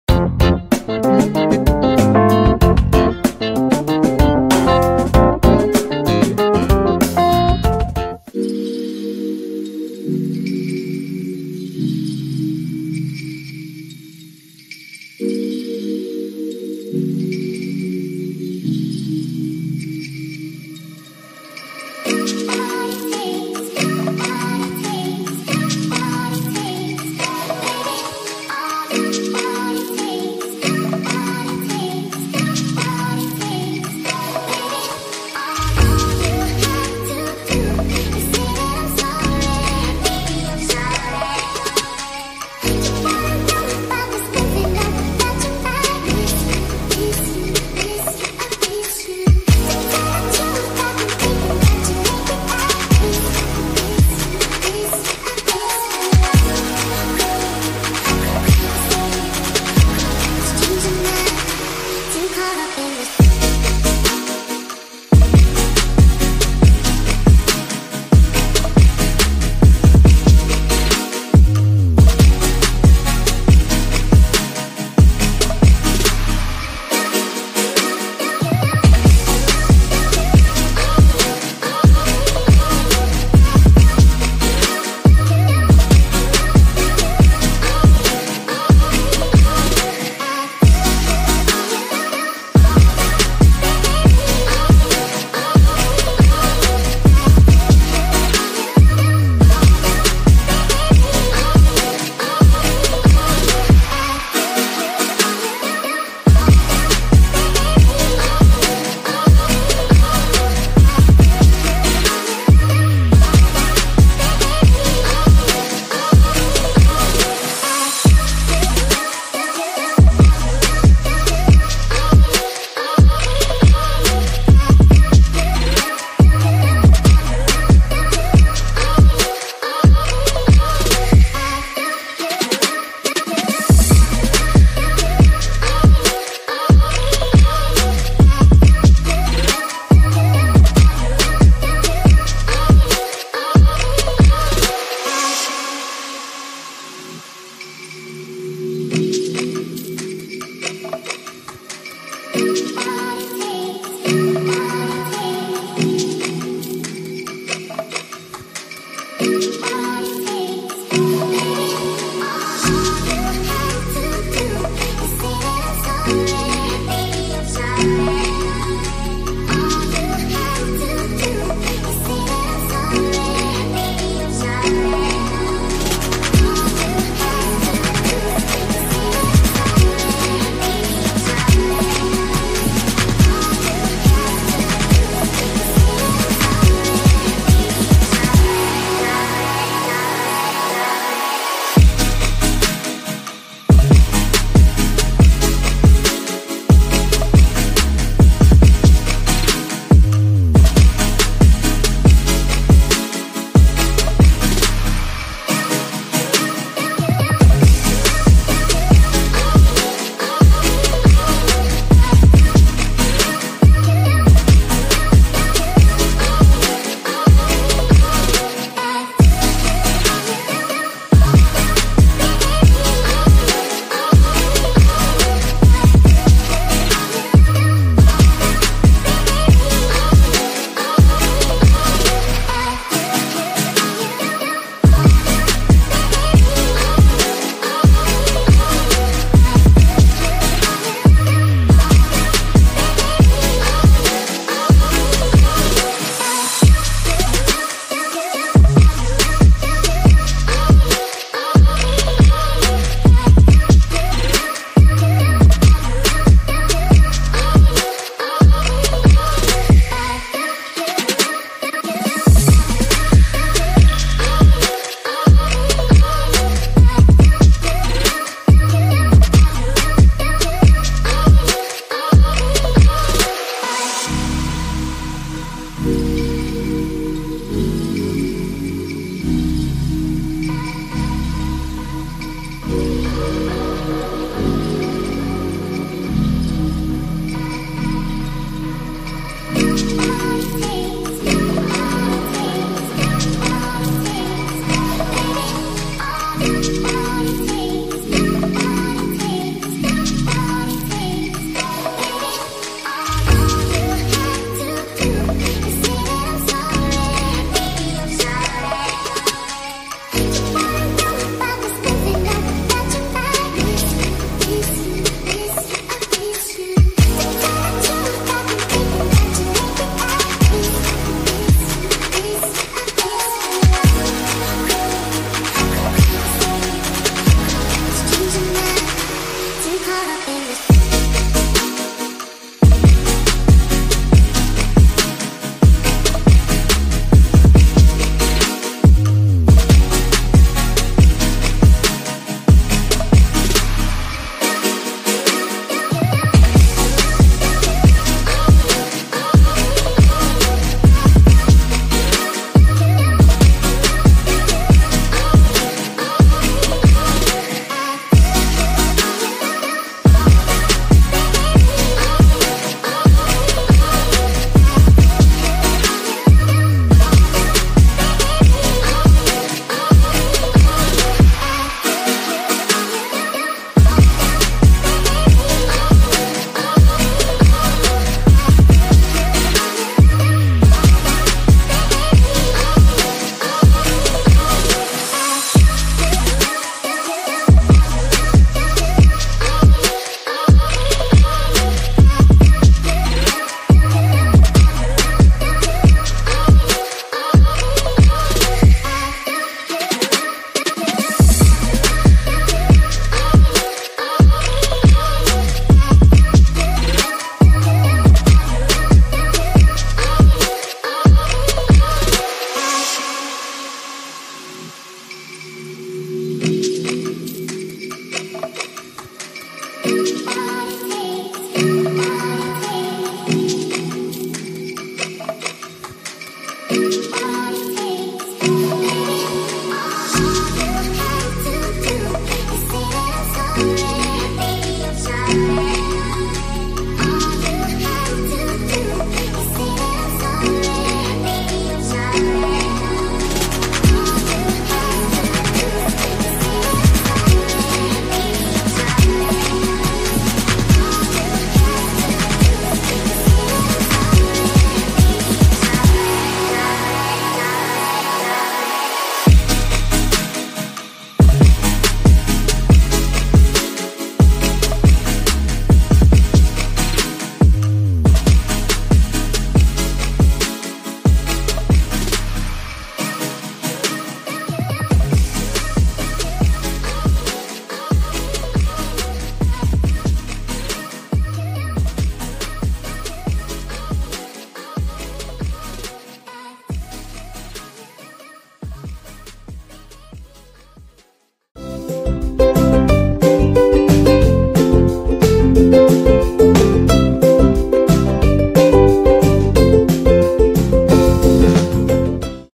KATALOG PROMO TUPPERWARE NOVEMBER 2016
30 Oktober - 26 November 2016

Assalamu'alaikum Baraya 🤗

Hari ini kita mau upload katalog lama
Untuk menemani kalian, sambil menunggu konten-konten terbaru dari kami

Admin mulai gabung tupperware dari bulan Oktober 2014, jadi katalog lama akan di up dari bulan itu ya 🤗

Pre order produk terbaru dapat hubungi admin kami di WA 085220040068 (chat only)

SEMOGA BERMANFAAT 🤗

Wa'alaikumsalam Wr. Wb.